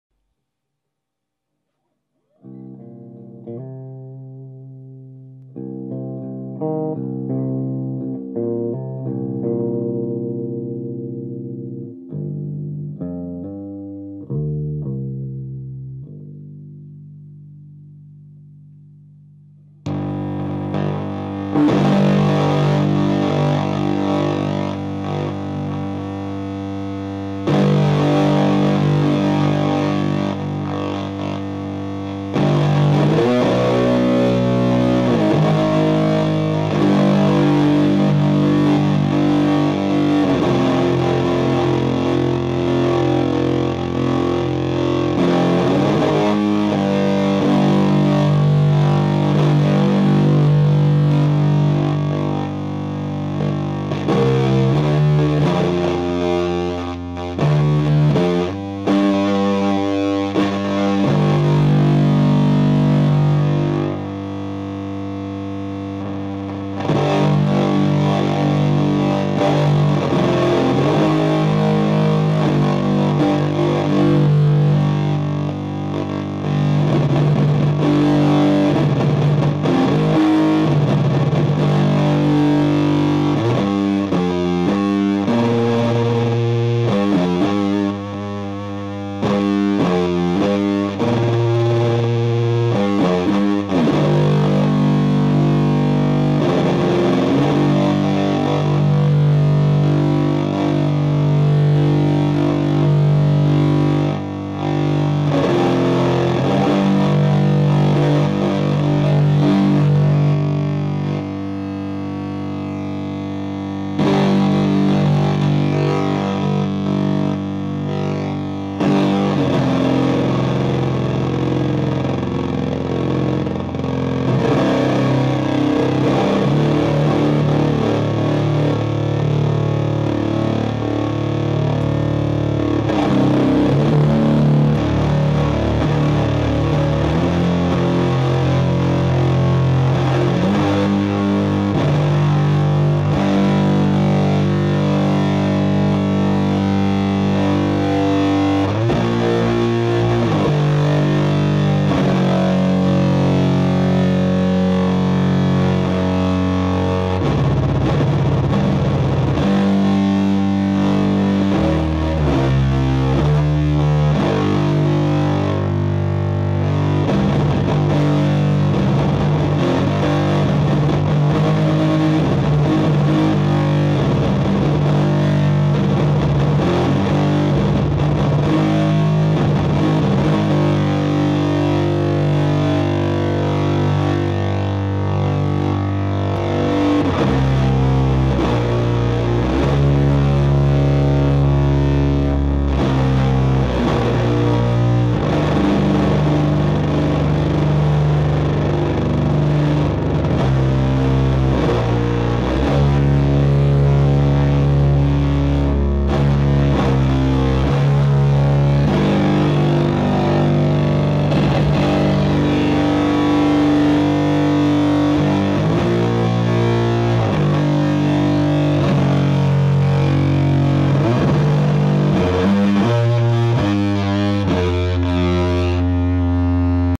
Triple Sunn Model T's (wear headphones)
2:15 added bass channel with 2x15
3:07 added Octave

OLP 30" Baritone tuned to G standard

Aculpuco Gold Clone(voiced to sound like a cranked Sunn Model T) into a Model feT( Sunn Model T pre Amp pedal)  into a Hovercraft Dwarvenaut (mine was voiced to a sound like a Sunn Model T)